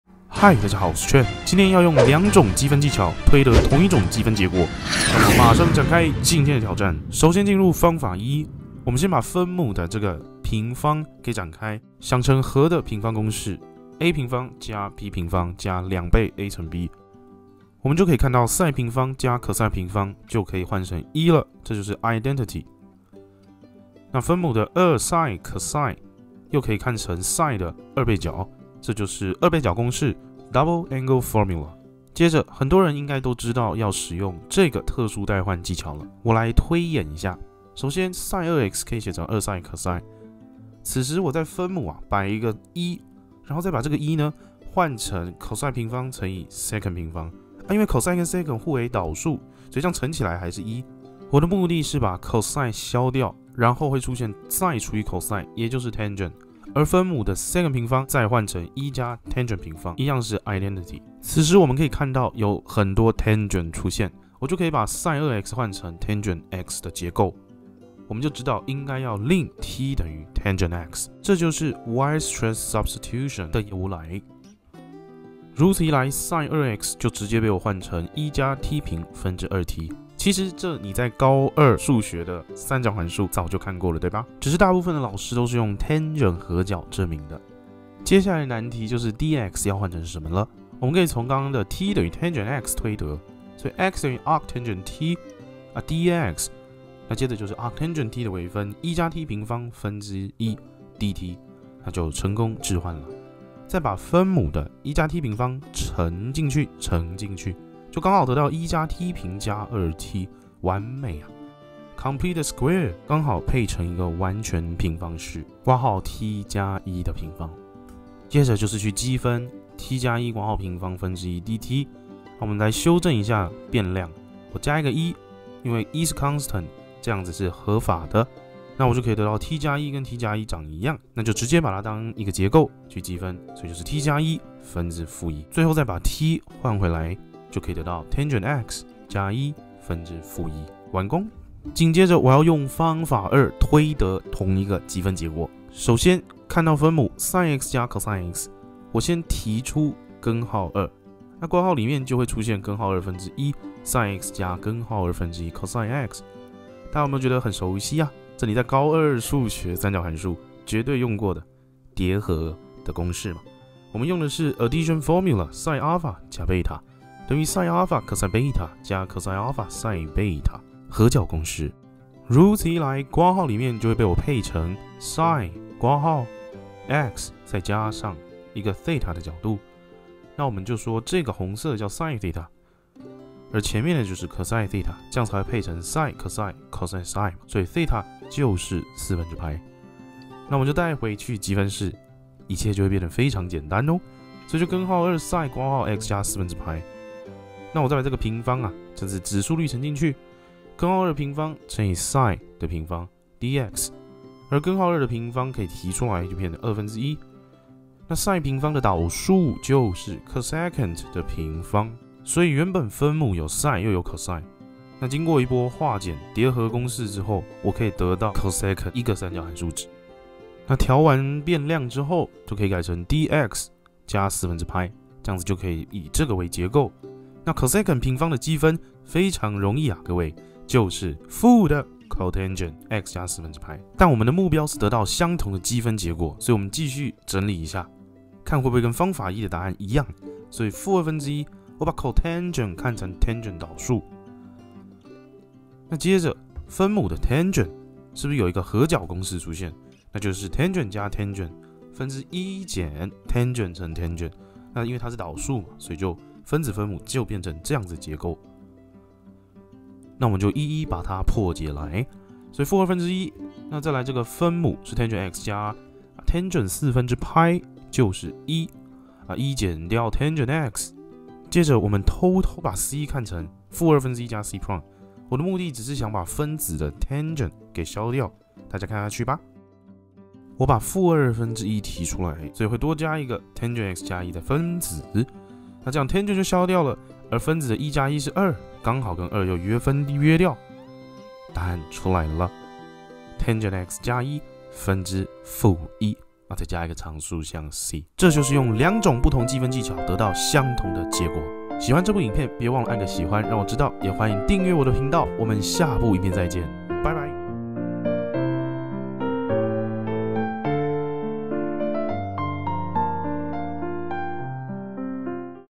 🔥1 / ( sin(x)+cos(x) )^2｜你可能沒想過的兩種積分技巧，推得同一種積分結果！竟然用到高中數學公式？
🐶1 / ( sin(x)+cos(x) )^2 積分，這題還有很多解法，那你會怎麼做？
留言區分享你的做法吧！

00:00  開場
00:06  方法一：特殊代換積分技巧 Weierstrass substitution (含完整推導)
02:22  方法二：高中數學三角函數和角公式 (Addition formula)、疊合

⬇️每日積分挑戰🔥Integral Everyday🔥每天會發一部「積分」算給大家看唷！